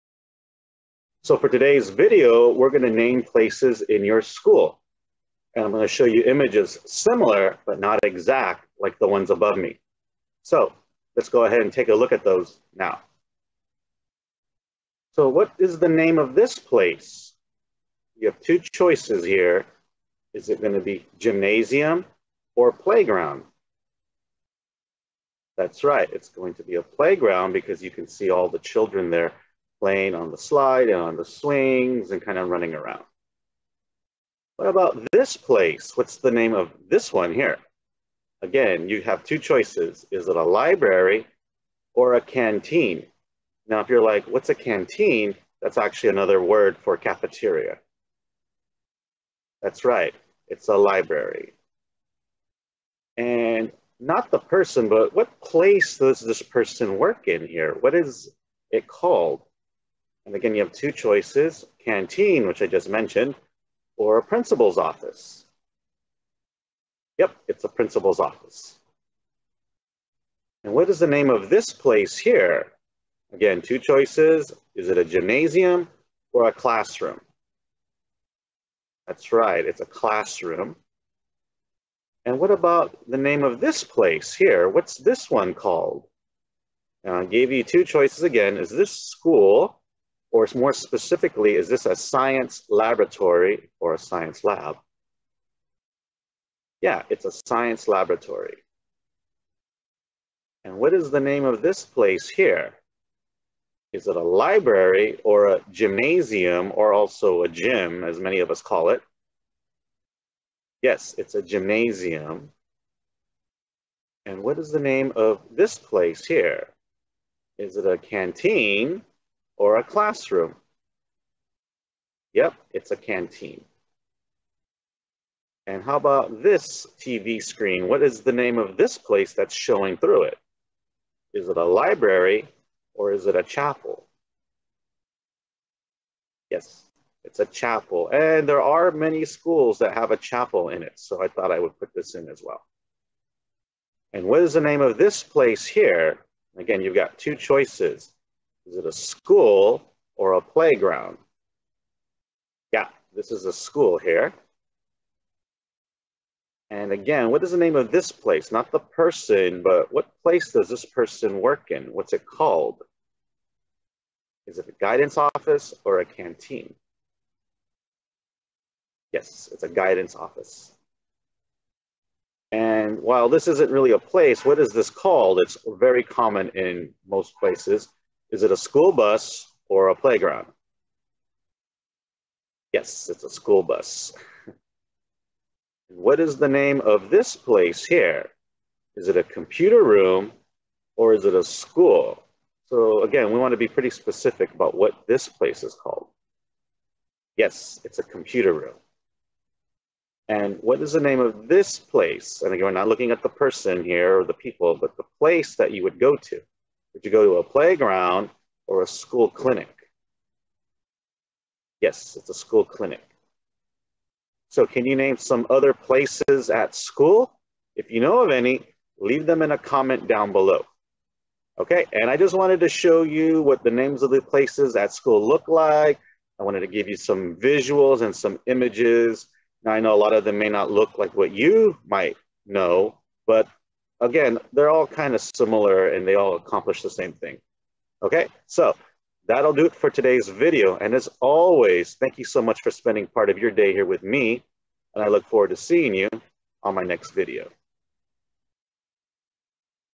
Name Different Places at School Animation Video
This video will go over naming places in school using animations which allows the viewer an opportunity to demonstrate listening and following directions by identifying various locations.

These videos are designed as a way to simulate what my tutorial sessions are like during remote learning that many students are now experiencing as part of their academics. Even if there is no remote learning taking place this video gives another opportunity for the viewer to have some extra practice with various subjects on math.